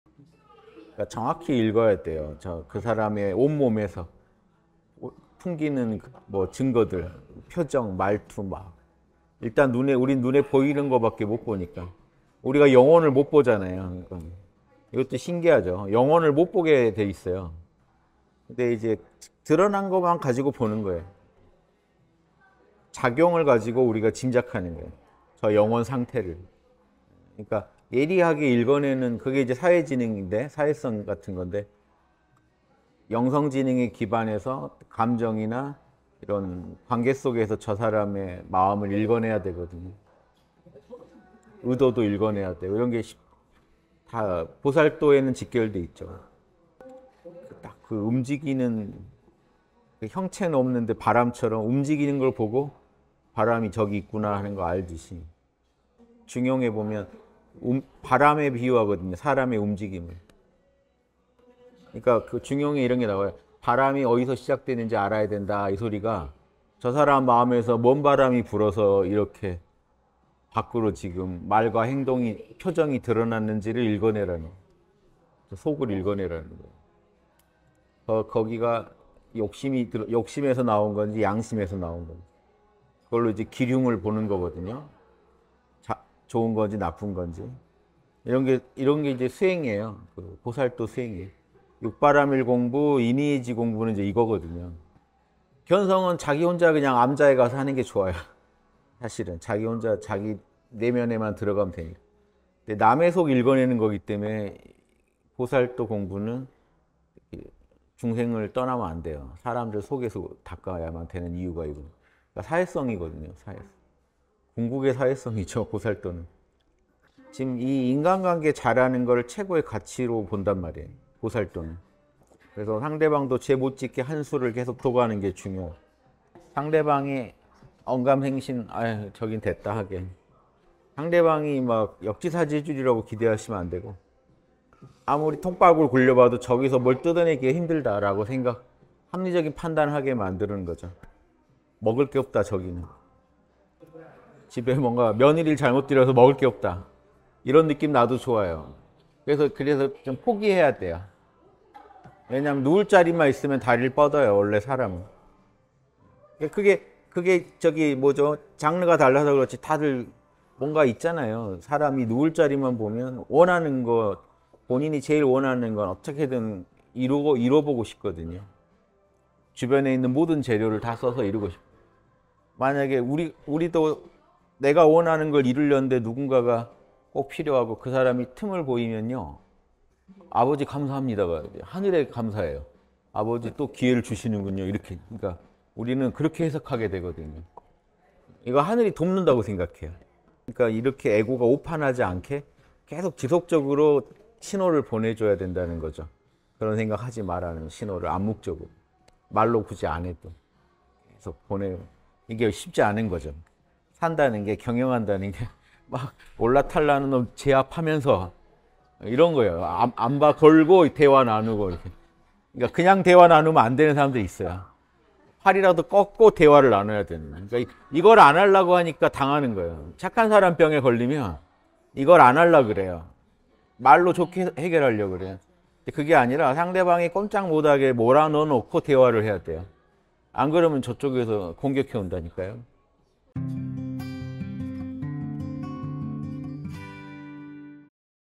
[생생톡톡] 상대의 마음을 읽고 제압하는 보살 _240616
정확히 읽어야 해요,
그 사람의 온몸에서
풍기는 증거들 표정, 말투.
일단 우리는 눈에 보이는 것밖에
못 보니까..
우리가 영혼을 못 보잖아요.
이것도 신기하죠.

영혼을 못 보게 돼 있어요.
그런데 이제 드러난 것만 가지고
보는 거예요. 작용을 가지고
우리가 짐작하는 거예요,
저 영혼 상태를.
그러니까 예리하게 읽어내는
그게 이제 사회지능인데,

사회성 같은 건데
영성지능에 기반해서
감정이나 이런 관계 속에서
저 사람의 마음을
읽어내야 하거든요.
의도도 읽어내야 해요. 이런 게 다
보살도에는 직결되어 있죠.

딱 그 움직이는 형체는 없는데
바람처럼 움직이는 걸 보고
바람이 저기 있구나
하는 걸 알듯이.
중용에 보면 바람에
비유하거든요, 사람의 움직임을.
그러니까 중용에 이런 게 나와요.

바람이 어디서 시작되는지
알아야 한다.
이 소리가, 저 사람 마음에서
뭔 바람이 불어서 이렇게
밖으로 지금 말과 행동이
표정이 드러났는지를
읽어내라는 거예요.

속을 읽어내라는 거예요.
거기가 욕심에서 나온 건지
양심에서 나온 건지.
그걸로 길흉을 보는 거거든요,
좋은 건지 나쁜 건지.
이런 게 수행이에요.
보살도 수행이에요.

6바라밀 공부, 인의예지 공부는
이거거든요.
견성은 자기 혼자 그냥 암자에
가서 하는 게 좋아요, 사실은.
자기 혼자 자기
내면에만 들어가면 돼요.
남의 속 읽어내는 거기 때문에

보살도 공부는
중생을 떠나면 안 돼요.
사람들 속에서 닦아야만 되는
이유가 있어요.
사회성이거든요, 사회성.
궁극의 사회성이죠, 보살도는.
지금 이 인간관계 잘하는 걸

최고의 가치로 본단
말이에요, 보살도는.
그래서 상대방도 죄 못 짓게
한 수를 계속 둬 가는 게 중요하죠.
상대방의 언감생심,
아 저긴 됐다 하게.
상대방이 막 역지사지해주리라고

기대하시면 안 되고
아무리 통밥을 굴려봐도 저기서
뭘 뜯어내기가 힘들다고 생각,
합리적인 판단을 하게 만드는 거죠.
먹을 게 없다, 저기는.
집에 뭔가 며느리를 잘못 들여서
먹을 게 없다.

이런 느낌 나도 좋아요.
그래서 포기해야 돼요.
왜냐면 누울 자리만 있으면
다리를 뻗어요, 원래 사람은.
그게 장르가 달라서 그렇지
다들 뭔가 있잖아요.
사람이 누울 자리만 보면 원하는 거

본인이 제일 원하는 건 어떻게든
이루고 이뤄보고 싶거든요.
주변에 있는 모든 재료를
다 써서 이루고 싶어요.
만약에 우리도 내가 원하는 걸
이루려는데 누군가가 꼭 필요하고
그 사람이 틈을 보이면요

아버지 감사합니다.
하늘에 감사해요.
아버지 또 기회를
주시는군요, 이렇게. 그러니까
우리는 그렇게 해석하게 되거든요.
이거 하늘이 돕는다고 생각해요.
그러니까 이렇게 에고가

오판하지 않게 계속 지속적으로
신호를 보내줘야 한다는 거죠.
그런 생각 하지 말라는 신호를
암묵적으로, 말로 굳이 안 해도
계속 보내요.
이게 쉽지 않은 거죠,
산다는 게, 경영한다는 게

올라탈라는 놈 제압하면서.
이런 거예요,
암바 걸고 대화 나누고.
그냥 대화 나누면
안 되는 사람들이 있어요.
팔이라도 꺾고 대화를 나눠야
하는 거예요. 이걸 안 하려고 하니까
당하는 거예요.

착한 사람 병에 걸리면
이걸 안 하려고 그래요.
말로 좋게 해결하려고 그래요.
그게 아니라 상대방이 꼼짝 못 하게
몰아넣어 놓고
대화를 해야 해요. 안 그러면
저쪽에서 공격해 온다니까요.

240616 생생톡톡(양덕선방 카페대담)

- 유튜브 Q&A LIVE  목20시·토16시·일17시 (월2회 견콘 있는 토요일 휴방)